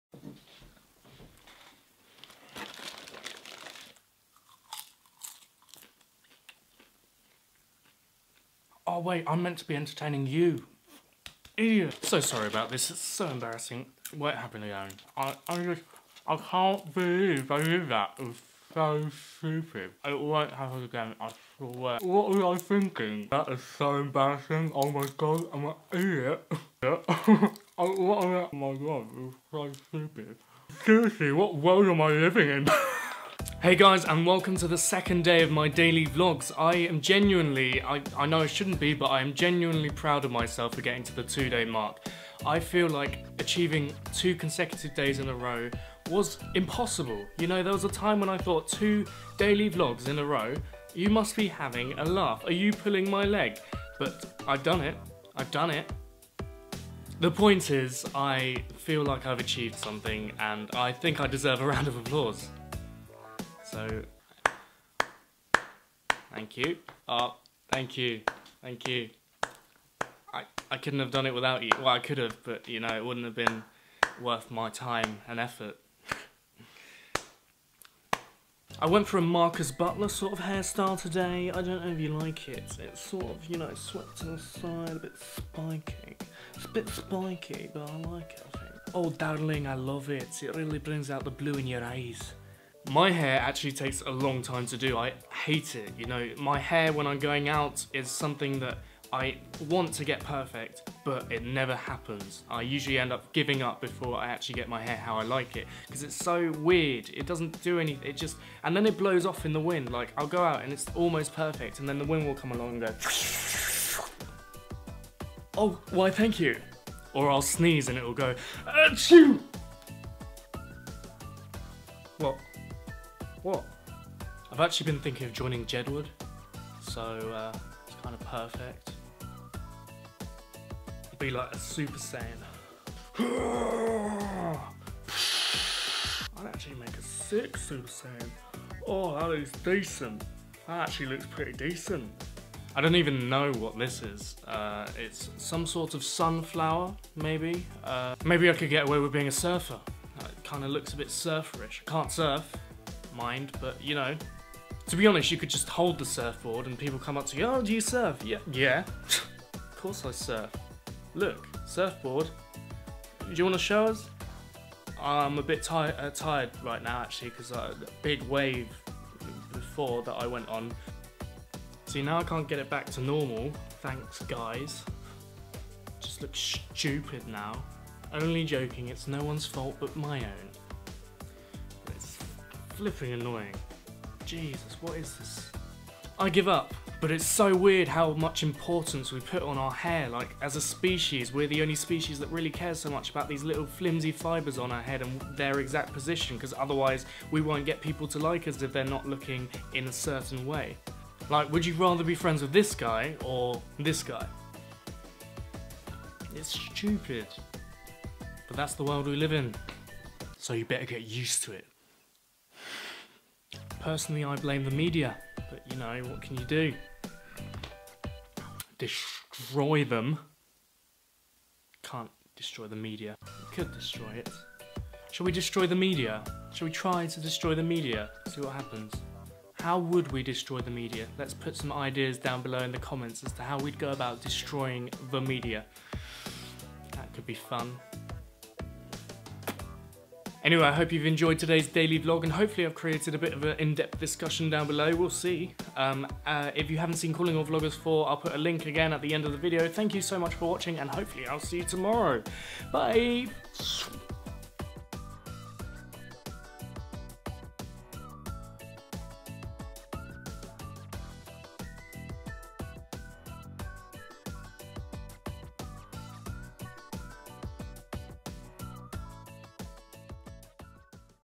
Horrendous Hair! (Daily Vlog #2)
I've made it to my second daily vlog! And in doing so I've found out that my hair does some very strange things... It's very annoying when I want to go out and look normal. Anyway, I hope you enjoy the video!